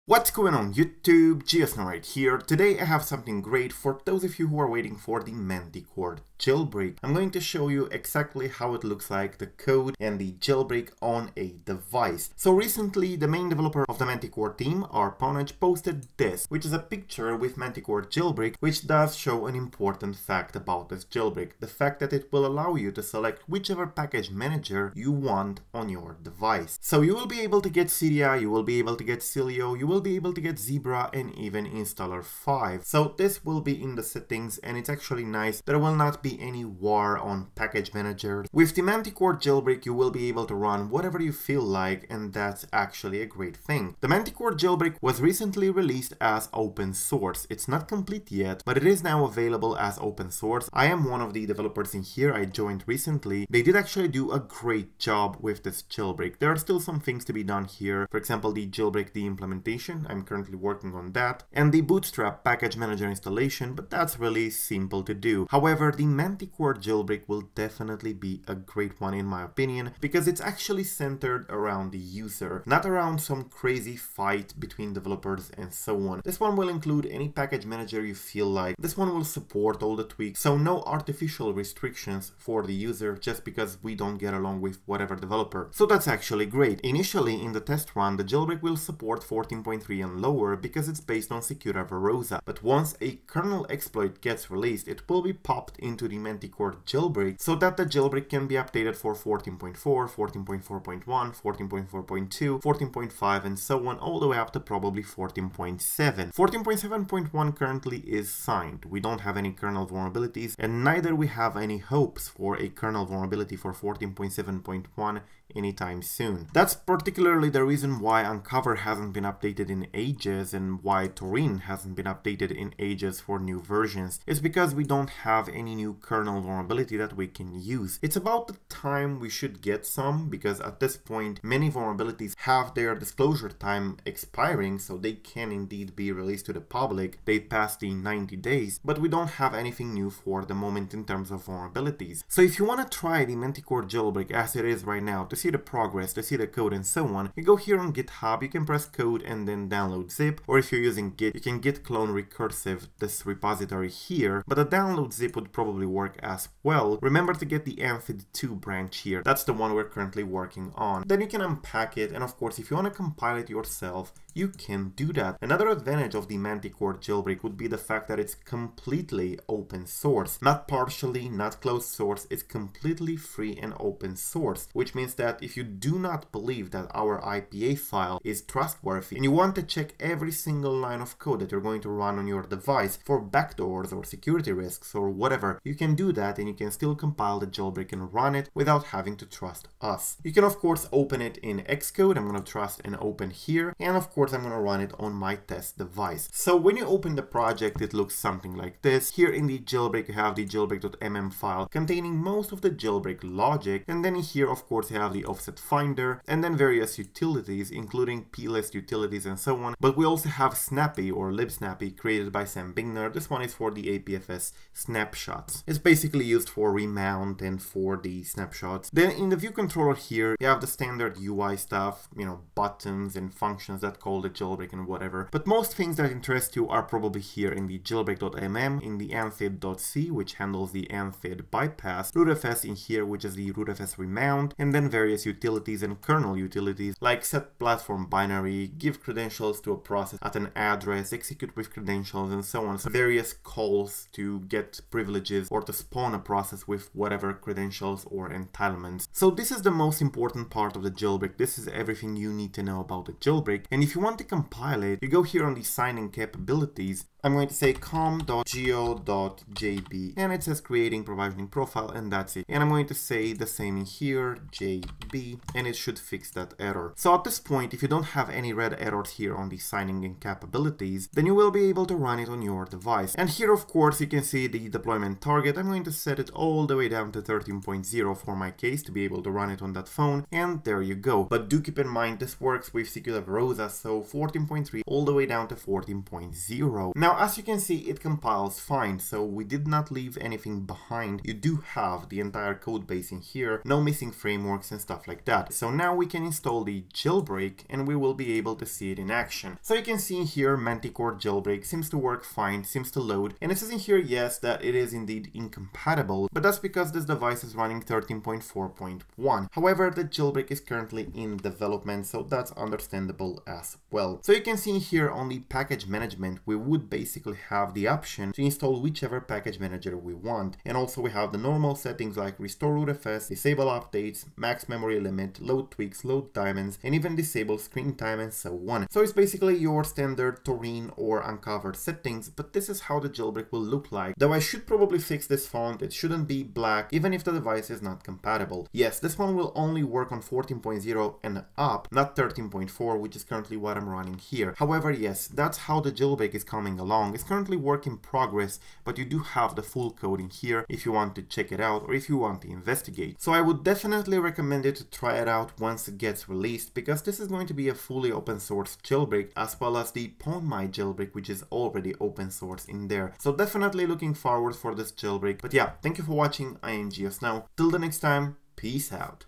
Manticore JAILBREAK (A12+) Great NEWS: Install Any Package Manager + Demo On Device
In this video, we're discussing some great news about Manticore Jailbreak, which will now allow the user to install any of the popular package managers, rather than being limited to just Cydia, Sileo, or Zebra. As progress is being made, a new UI update was pushed on GitHub which allows the user to select the package manager to be installed once the jailbreak process is complete, making Manticore the only jailbreak to bundle all available package managers.

Amongst the currently supported options, we have Sileo, Cydia, Installer 5, and Zebra, all of them being popular and having their own user base. Manticore Jailbreak is fully open source which allows anybody to contribute to the project, compared to jailbreaks like Unc0ver which have become closed source and heavily obfuscated.

~ GeoSn0w